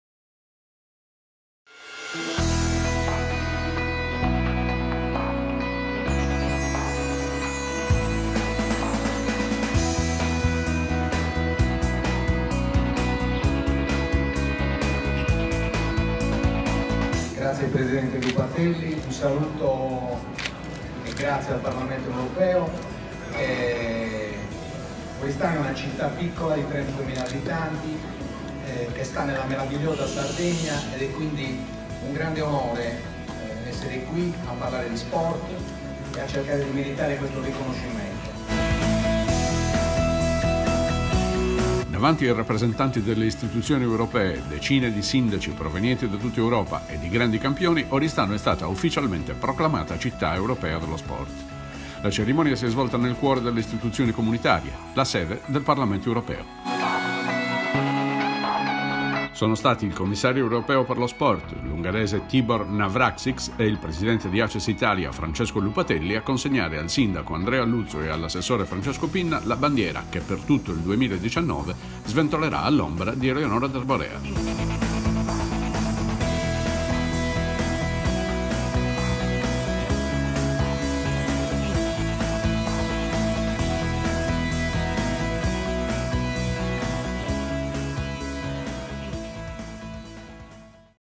oristano città europea dello sport
Il 4 dicembre a Bruxelles Oristano è stata insignita del titolo di Citta Europea dello Sport. Il Sindaco di Oristano e l'Assessore dello Sport Francesco Pinna hanno ricevuto la bandiera di Città dello Sport dal Commissario Europeo Tibor Navracsics e dal presidente di Aces Italia Francesco Lupatelli